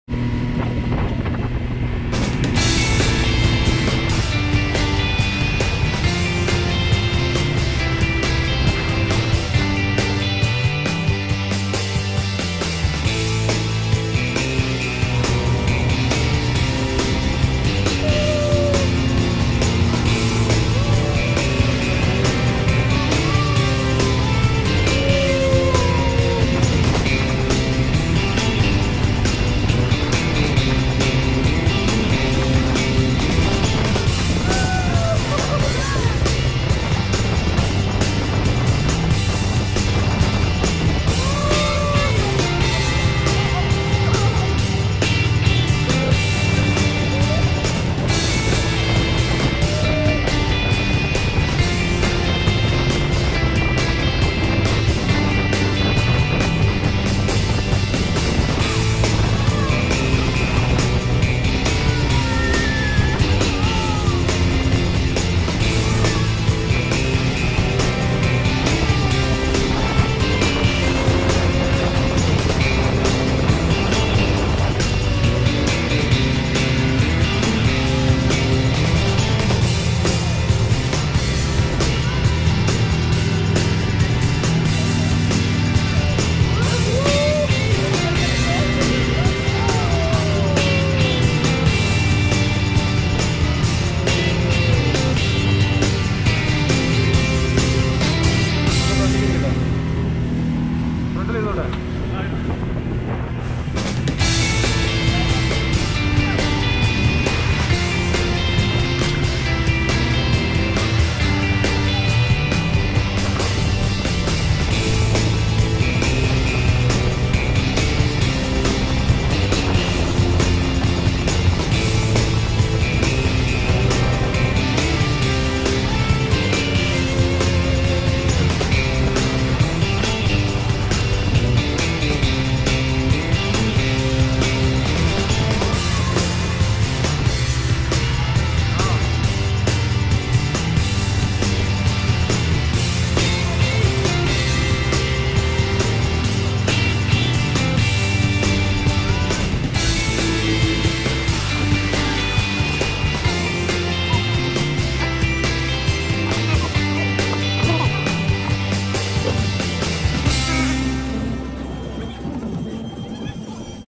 Speed boat ride - Alleppey
A thrilling short speed boat ride at Alleppey terminal
A must try experience
Charges as of today - 500/- for a round
Per hour - 3500/-